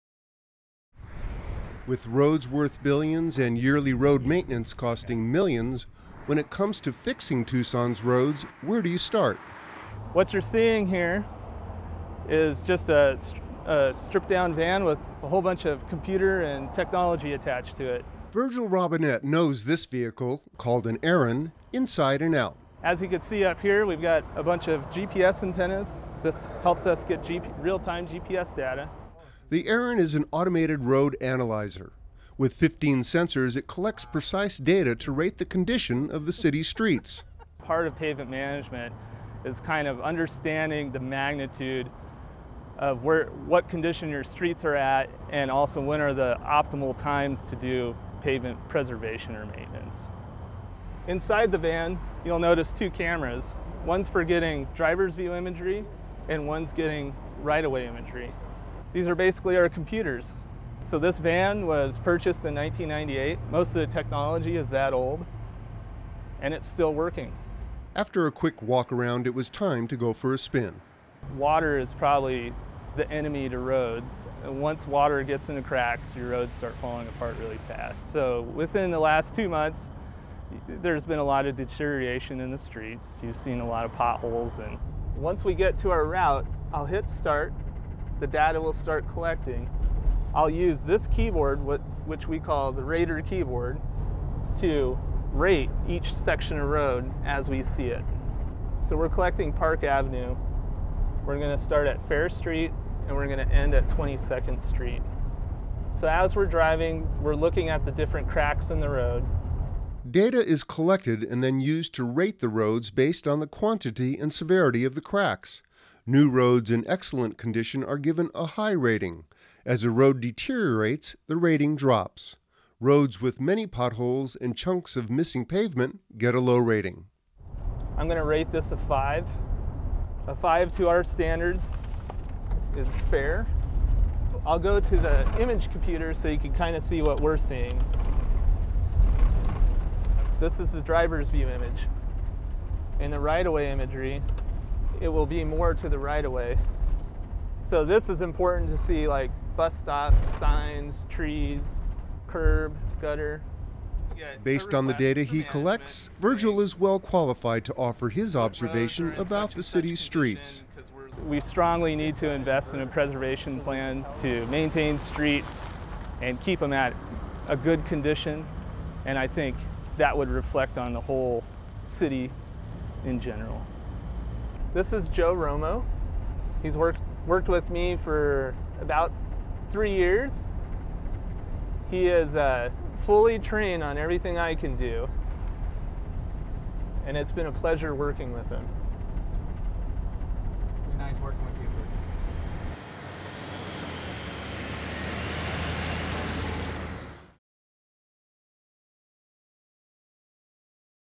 More Than a Van
The ARAN is an automated road analyzer that collects data to rate the condition of Tucson's streets. Produced by Tucson 12.